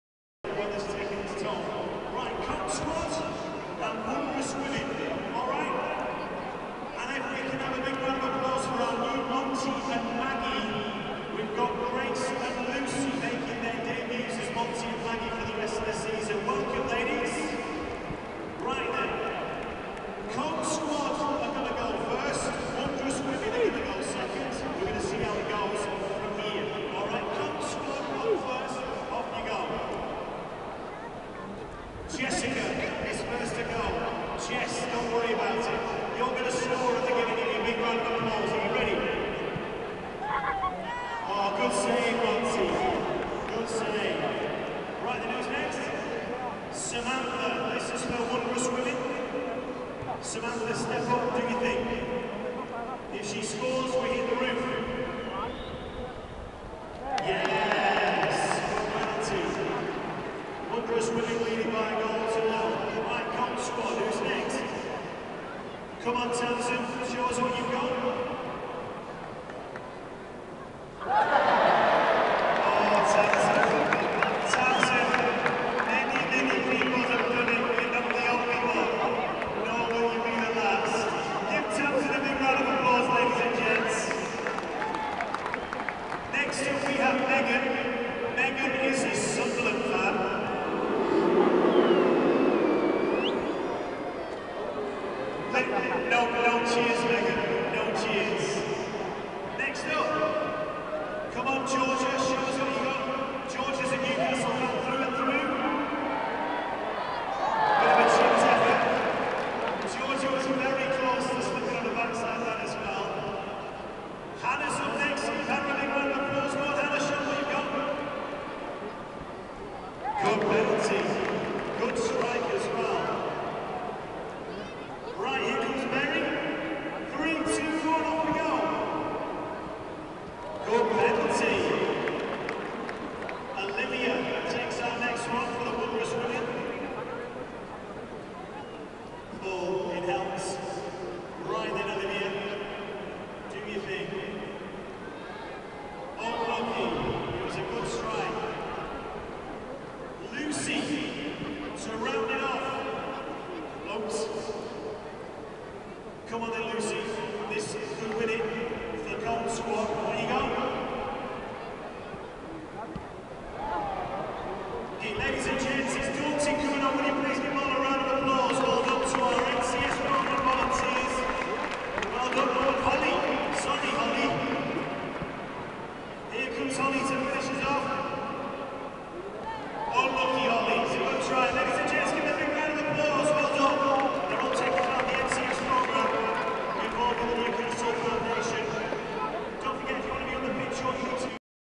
Pre-Season Friendly, Newcastle United 2 - 0 Hellas Verona played Sunday 6/8/17
match highlights Half - Time entertainment penalty kicks,Wondrous Women  v Goal Squad .